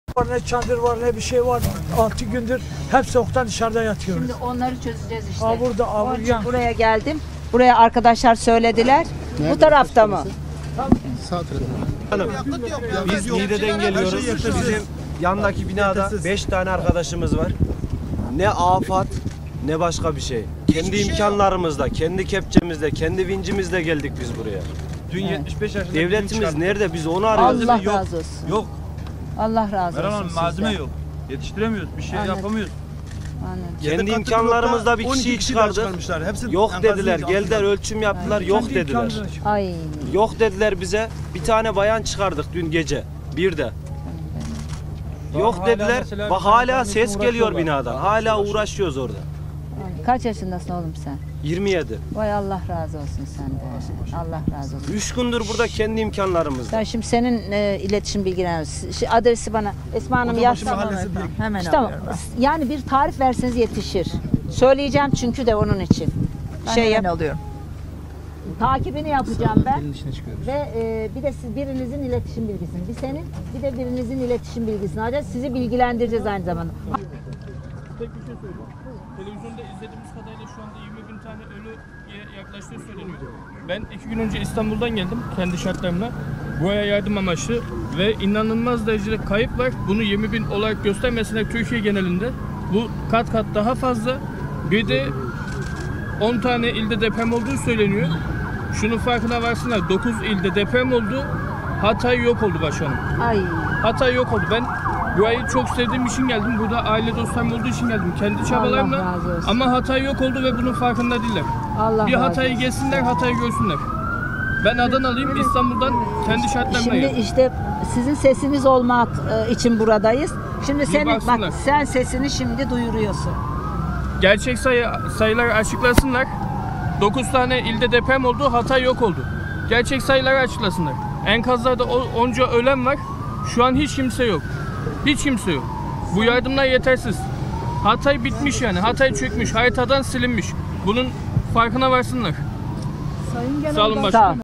Mucizelerin gerçekleşmesi söz konusu.
İYİ Parti Genel Başkanı Meral Akşener, Antakya deprem bölgesinde; “Hala enkazlardan canlı çıkıyor.
 Dolayısıyla, arama kurtarma, dinleme faaliyetlerinin bitirilmemesi lazım.
 Dolayısıyla mucizelerin gerçekleşmesi söz konusu. Bunun devamı lazım” dedi.